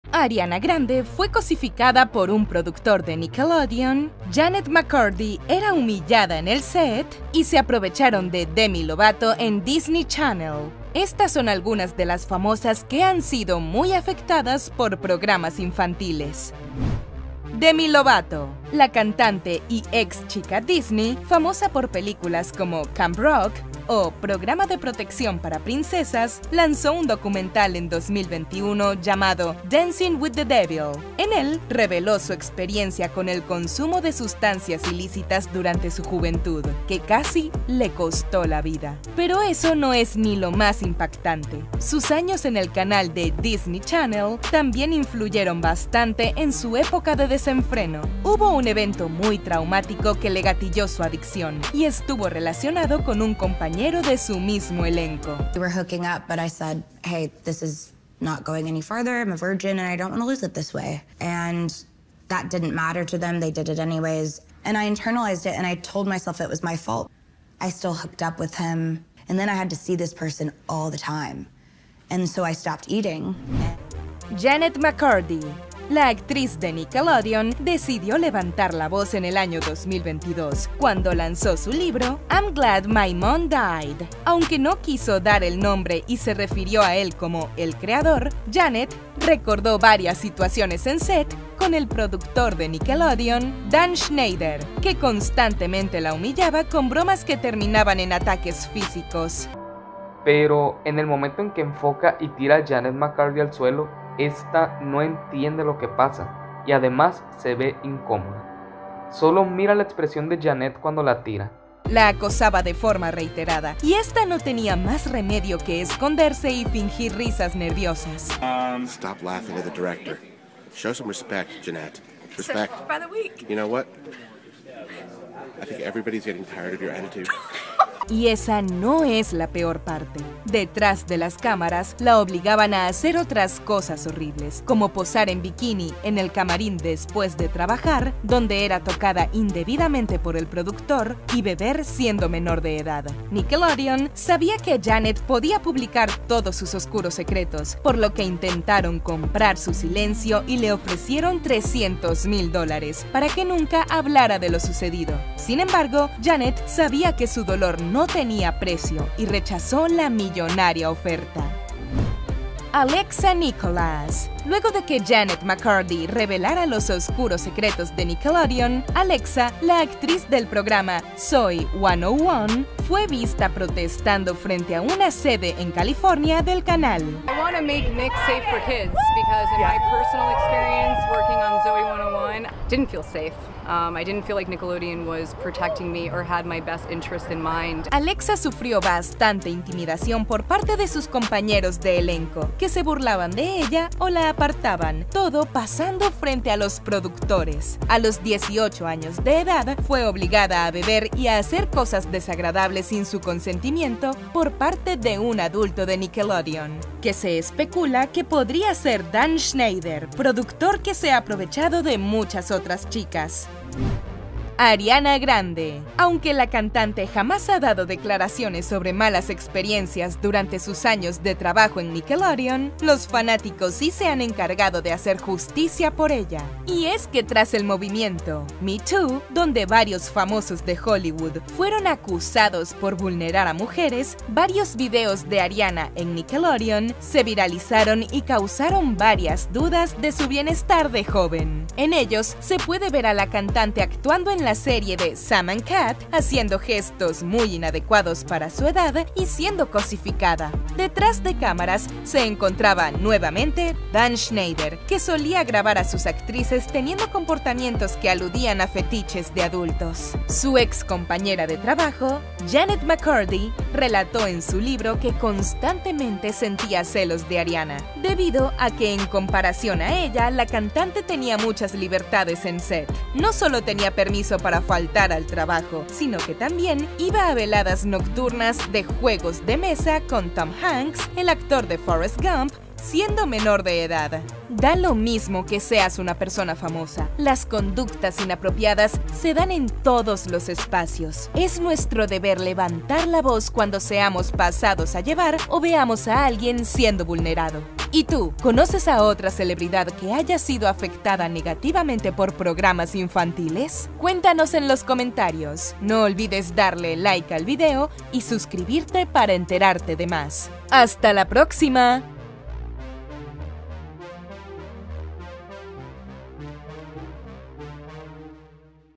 Famosas afectadas por programas infantiles | Showbiz
Ariana Grande fue cosificada por un productor de Nickelodeon, Jennette McCurdy era humillada y degradada en set y se aprovecharon de Demi Lovato en Disney Channel. Estas son algunas de las famosas que han sido afectadas por programas infantiles.

¿Te gustó nuestro canal? Cuéntanos sobre ti